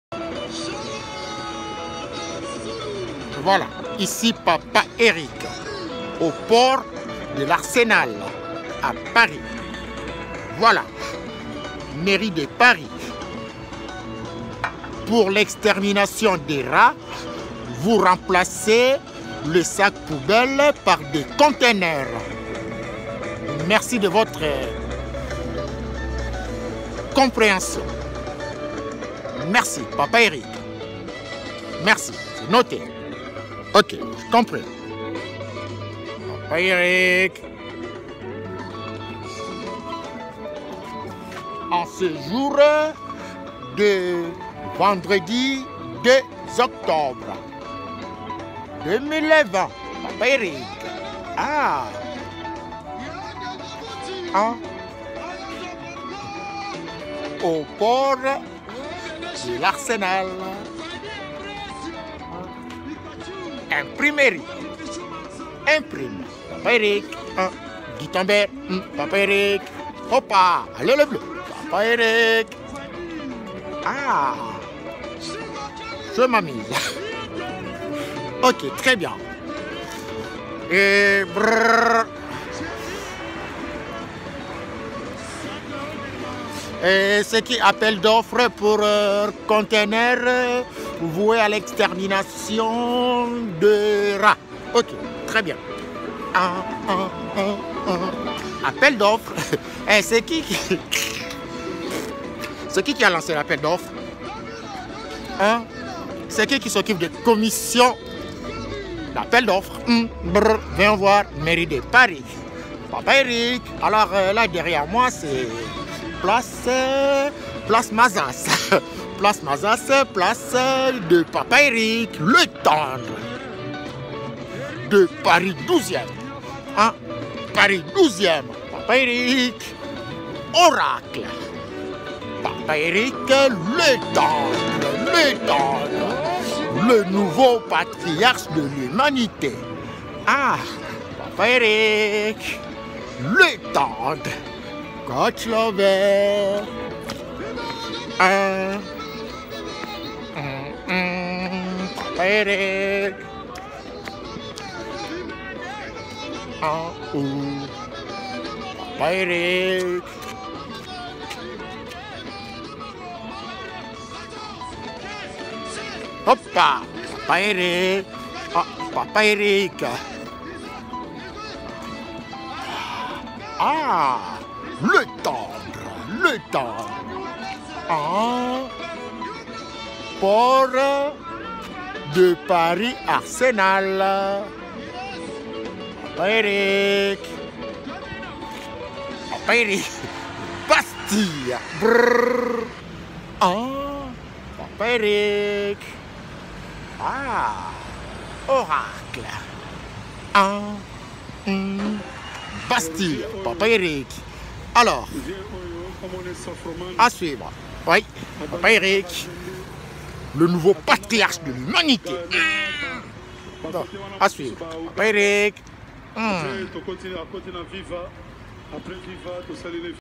PAPA - ÉRIC AU JARDIN DU PORT DE L’ARSENAL !!!
* CE VENDREDI 2 OCTOBRE 2020
* PAPA - ÉRIC S’AMUSE
* AU JARDIN DU PORT DE L’ARSENAL
* À LA BASTILLE !
* VIVE PAPA - ÉRIC LE TENDRE !!!